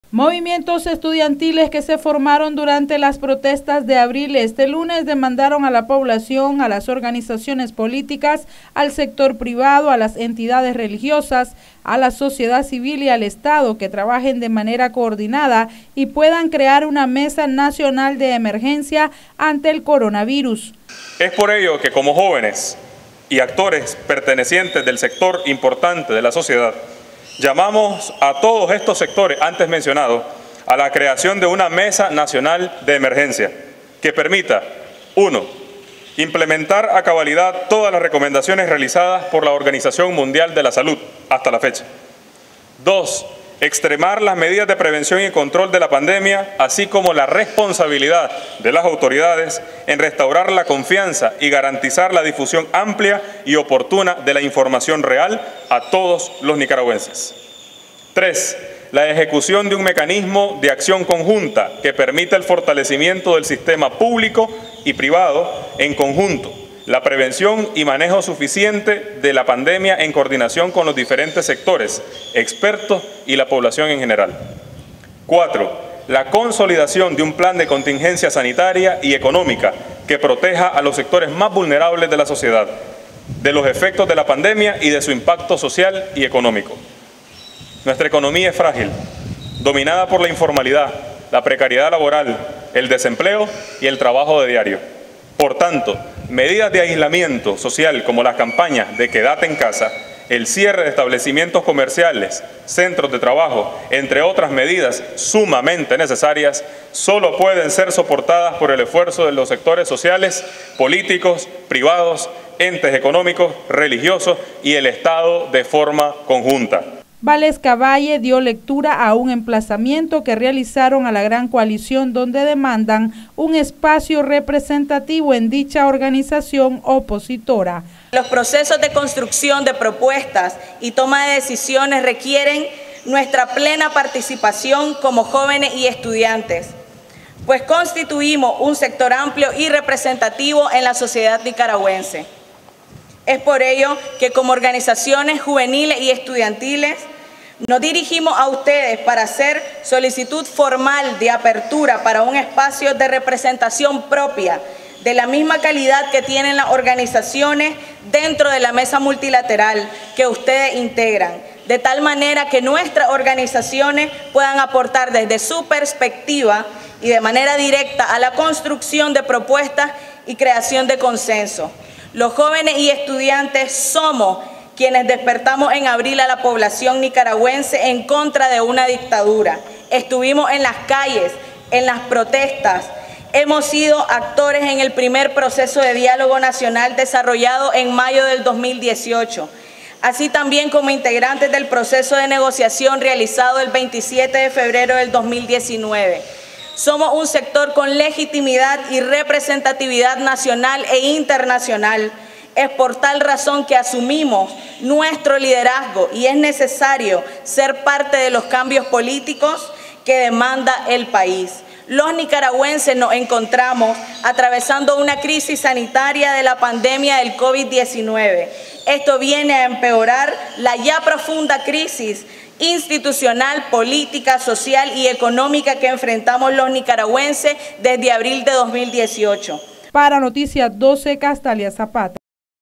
Mov. Estudiantiles demandan creación de una mesa nacional de emergencia
Movimientos estudiantiles demandan se conforme una mesa nacional de emergencia y a la vez solicitaron a la gran coalición, un espacio de representación propia.